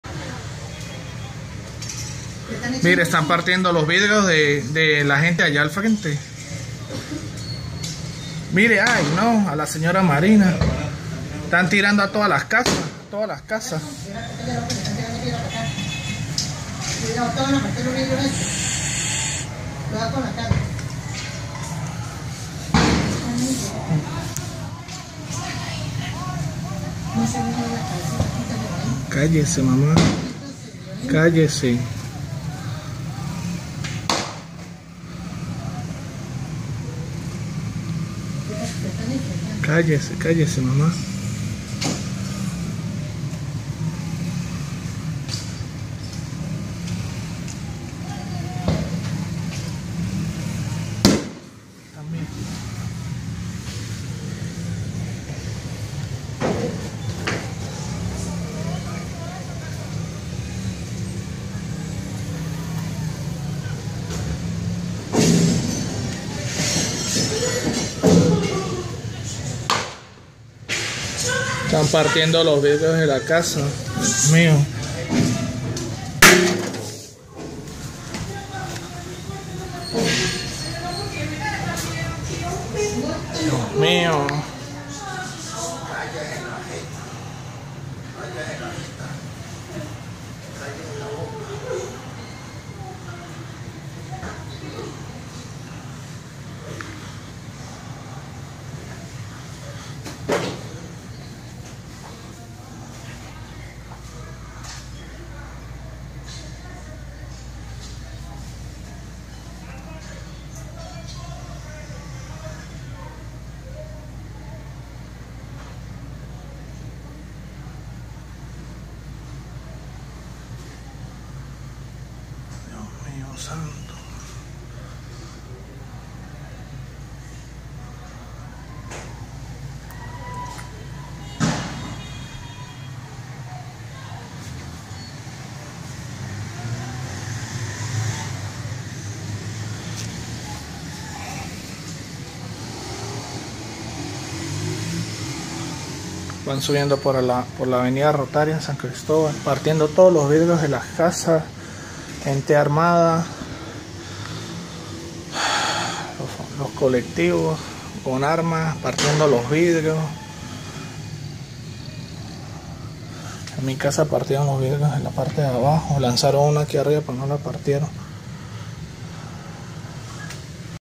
Caso Venezuela - malandros "colectivos" con armas en San Cristóbal rompen vidrios de casas
Video de Saúl Maldonado